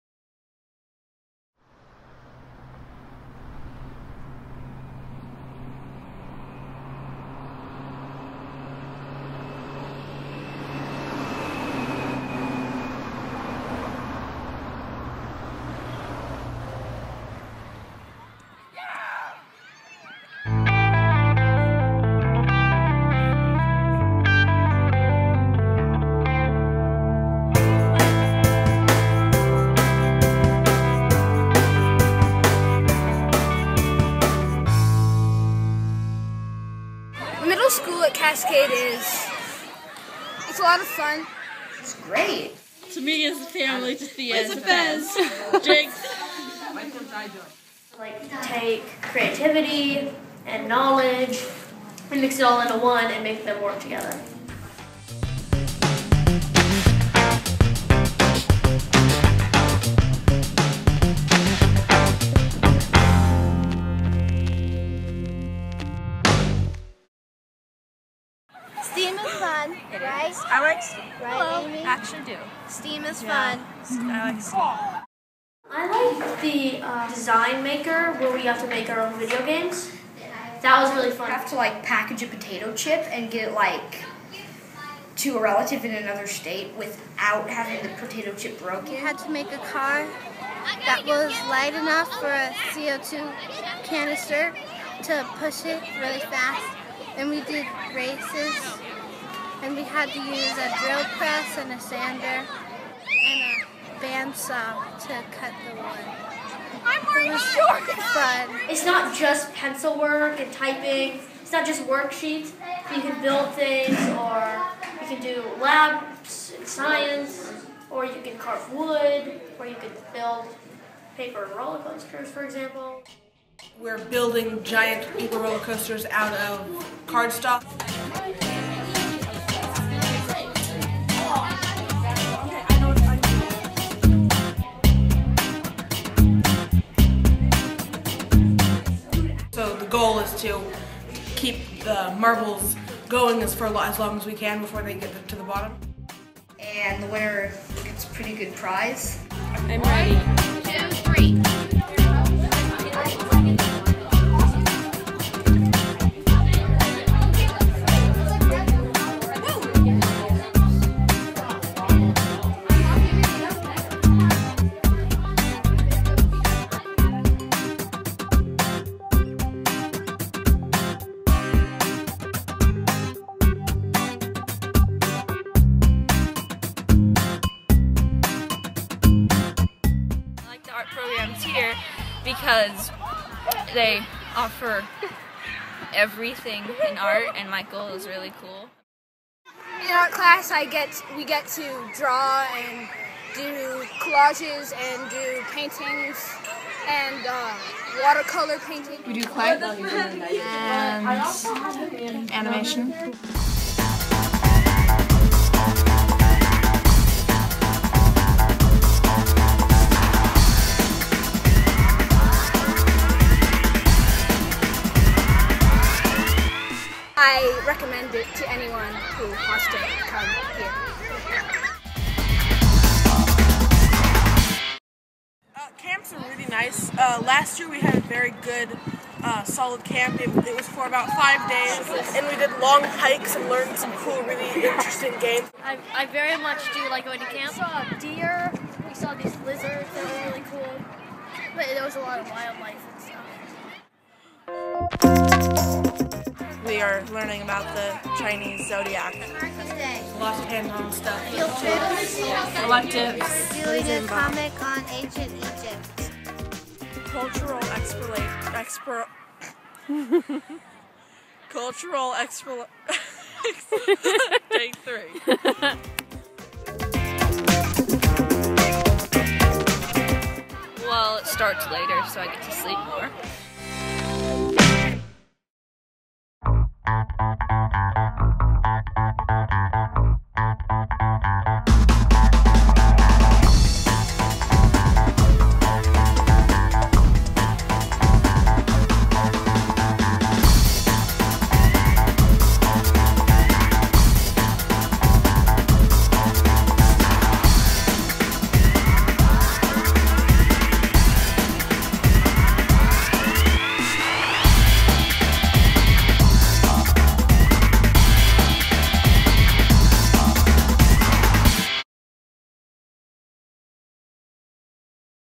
Shoreline's 3rd Middle School - Cascade K-8
Middle school at Cascade K-8 is small and intimate. Kids get a personalized and hands-on approach to academics, which enables them to build confidence and excel in their studies. Students enjoy the in-depth art program, the bi-annual outdoor education camps, and the STEAM program which weaves together science, technology, engineering, arts, and mathematics. This video was created by Cascade K-8 PTSA.